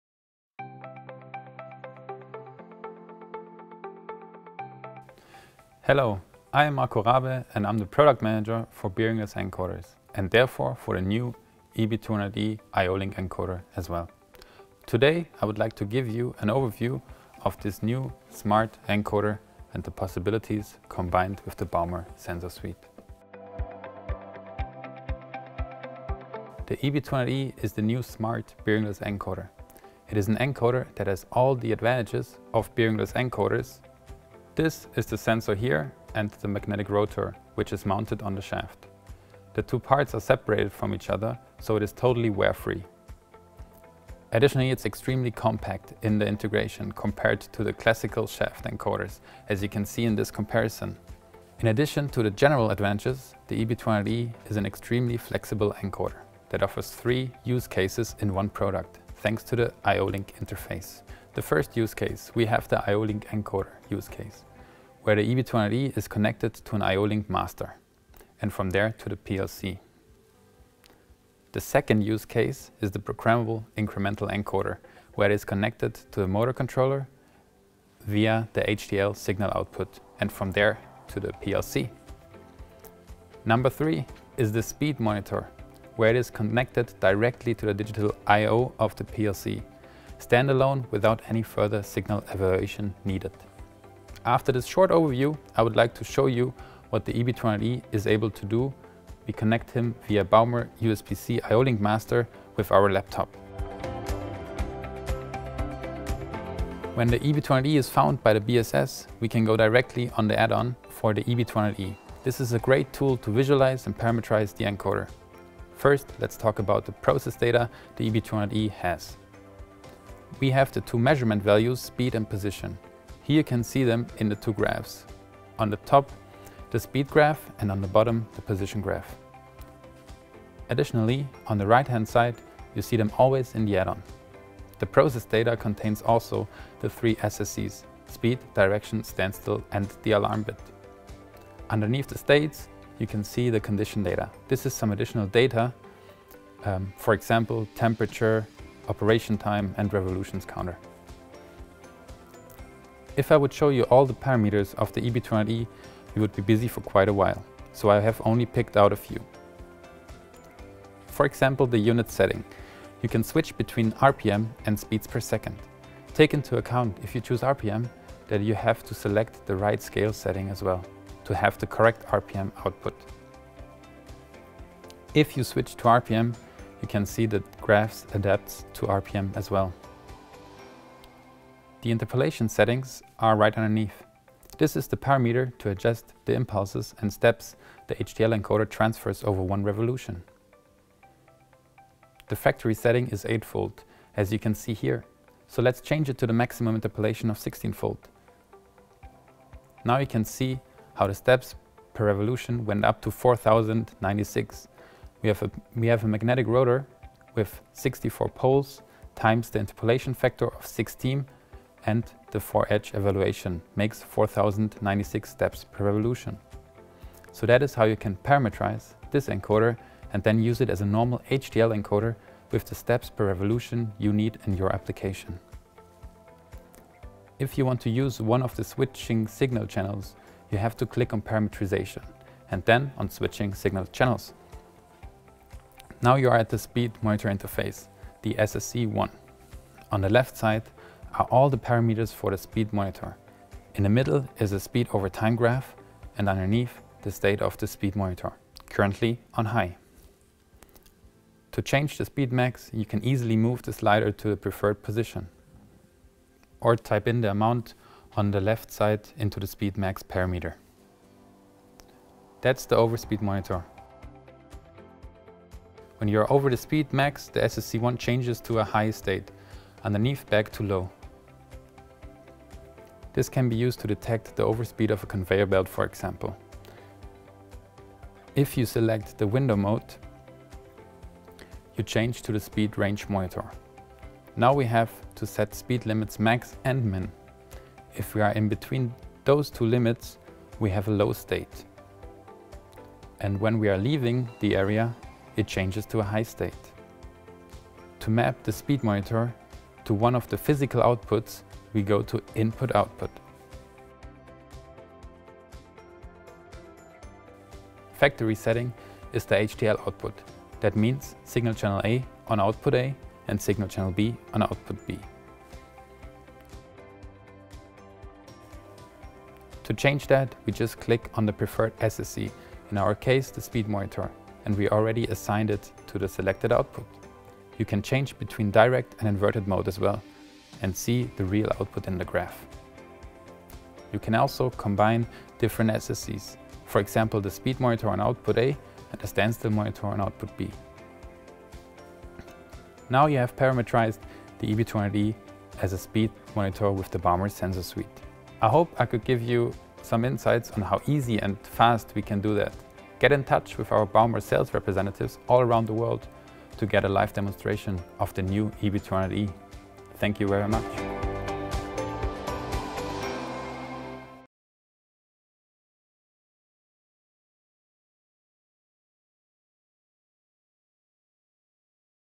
Baumer | EB200E encoder | Smart incremental bearingless encoder with IO-Link interface
Electrical and mechanical integration of encoders has become much easier and more economically viable. The new bearingless encoder EB200E from Baumer is proving this right. It can be compactly integrated on any axis without complex adaptation of the mechanics. EB200E is the first smart incremental bearingless encoder with IO-Link on the market. The interface reduces the cost and effort of cabling and a speed monitoring function replaces additional hardware components.

The IO-Link interface allows for up to 8 encoders to be connected to an IO-Link master by cost-effective standard cables. This way, EB200E opens up completely new areas of application: Encoder parameterization is via IO-Link. This interface enables parameterization of two switching outputs to act as a speed monitor for overspeed, speed range, standstill or direction of rotation. Furthermore, EB200E also delivers conventional HTL signals.

With a shallow installation depth of 12 mm and flexible shaft diameters, the bearingless encoder integrates into the machine design in a space-saving way. Thanks to non-contact sensing between sensor head and magnet ring, the encoder endures dirt, shock or vibration. Also the new Baumer Sensor Suite supports plant engineers as convenient PC tool for intuitive parameterization and data visualization.